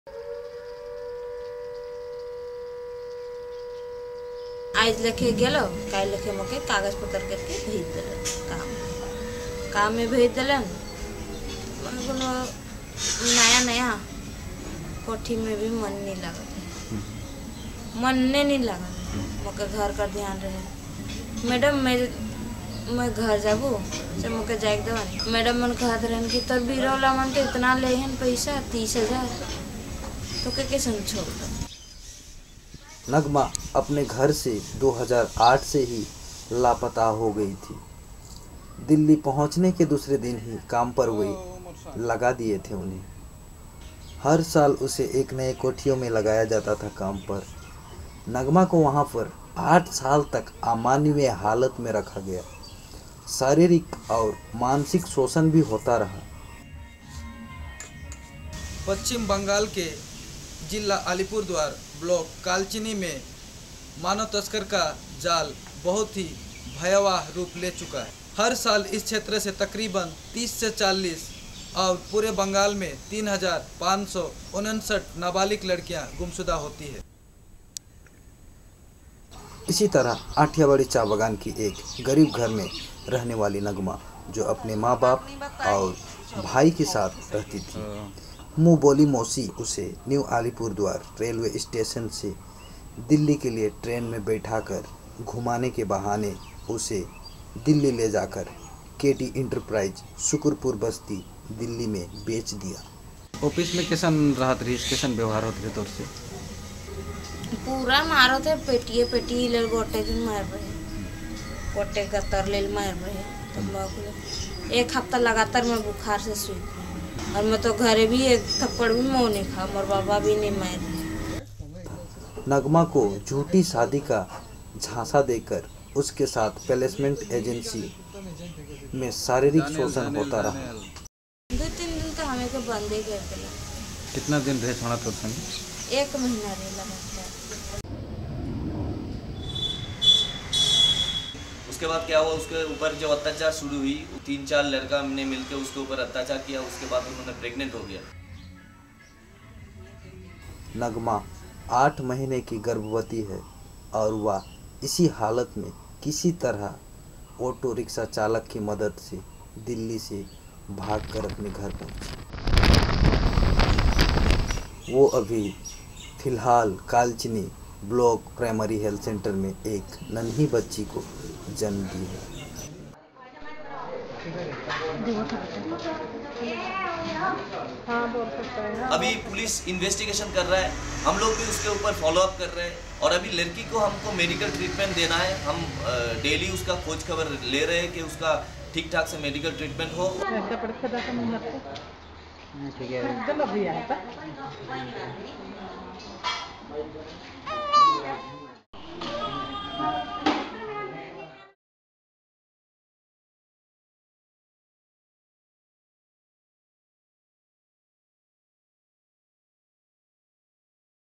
Trafficked at 11, Pregnant Teen Comes Home to Police Inaction
Harihar Nagbansi reports from Alipurduar, West Bengal, for Video Volunteers

Nagma has returned home after nine years. But this is hardly cause for celebration. In 2008, when she was only eleven, someone she called her aunt took her away on the pretext of a holiday and trafficked her. From a small village in a tea estate in Alipurduar district in West Bengal, she was taken all the way to New Delhi and sold to a placement agency for domestic workers. She was put to work in a new household every year and endured unimaginable physical and mental torture.